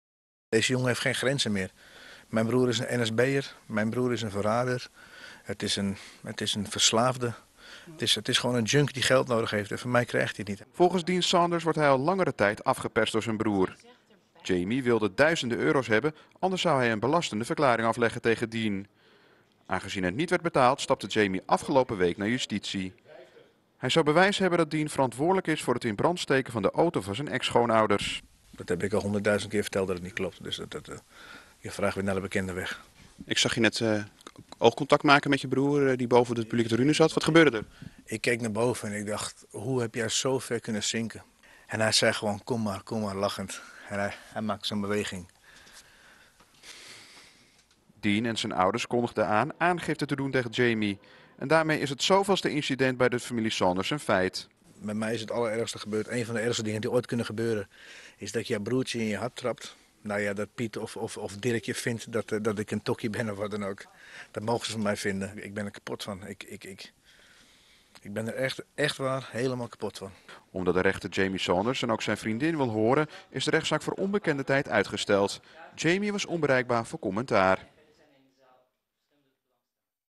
Dean Saunders: "Mijn broer Jamie is een beest dat opgesloten moet worden"
Zanger Dean Saunders moest vandaag in hoger beroep voor de rechter verschijnen vanwege brandstichting bij zijn ex-schoonouders en het mishandelen van zijn ex-vriendin. Maar na afloop had niemand het daar meer over. Het was vooral de belastende verklaring die zijn eigen broer Jamie tegen hem aflegde die voor commotie zorgde.